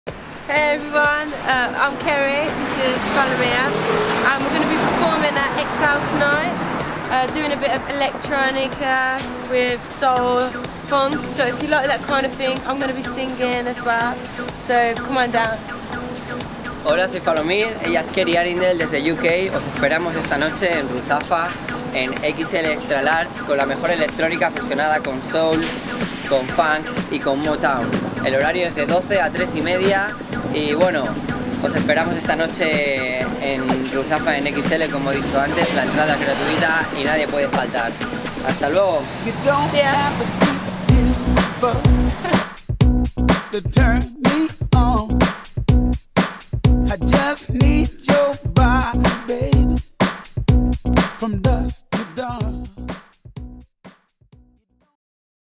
Keri Arrindell+Falomir! CTDSfilms .mp4
Keri Arrindell y Falomir! presentan una noche temática llena de Soul donde la cantante inglesa interpretará covers al ritmo de los beats de Falomir!. Sonarán artistas como Diana Ross, Moloko, Jamiroquai, Donna Summer, Amy Winehouse, Marvin Gay or Prince. Let's Groove!